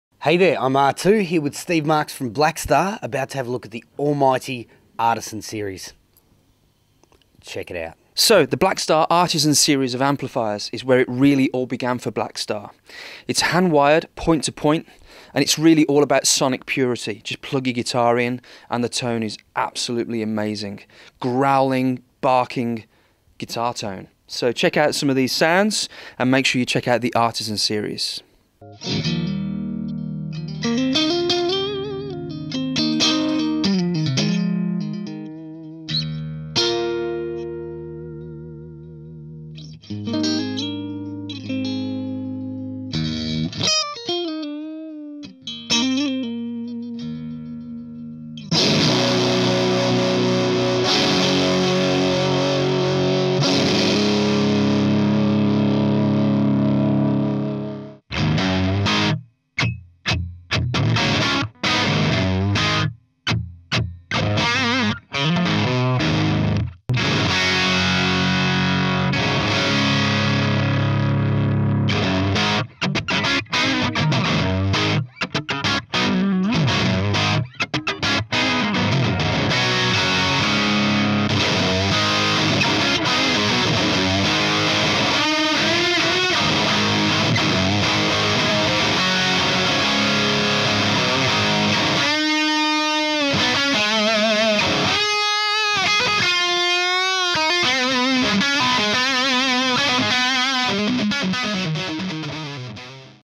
Blackstar Artisan 30 Head with Steve from Blackstar UK
The award-winning Artisan 30 in a head format. The Artisan 30H takes the magical sonic qualities of the 30W EL84 power stage and distils it to a new level of tonal purity. One channel is built around an EF86 with a 5-way Bass Shape switch and the other an ECC83 with the addition of a Voice control for extra tonal flexibility. The Master Volume delivers the pure, natural crunch that you would expect from an amp of this quality.